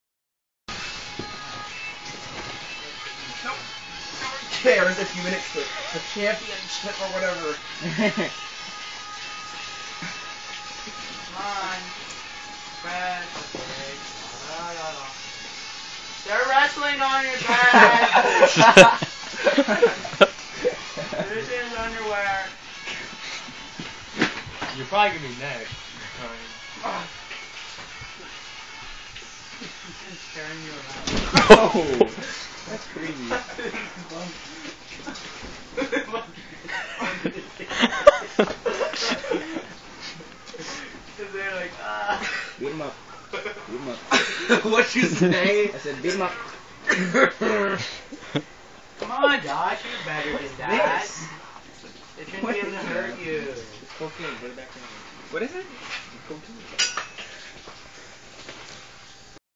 Teen wrestles another teen to the bed
Was at camp and just had to record this